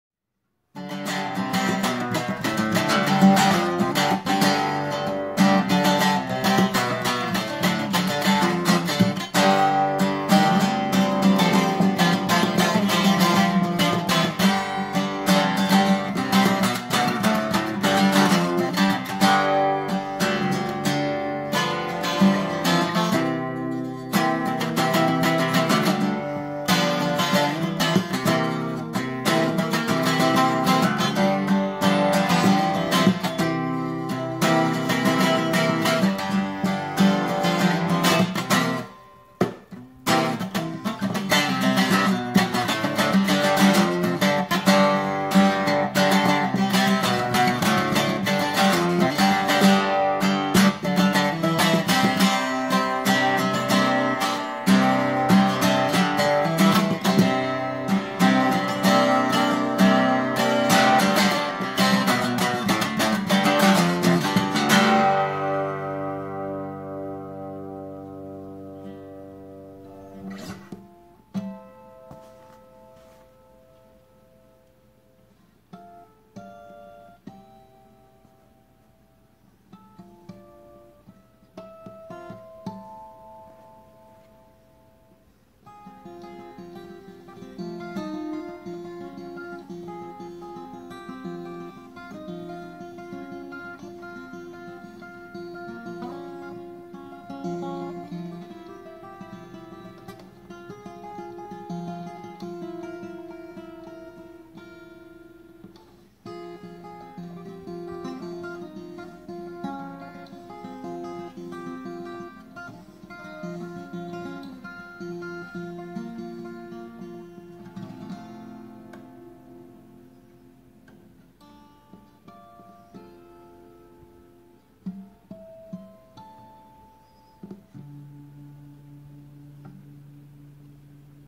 Twelve Moods
Playing the EKO 12. Two short ideas.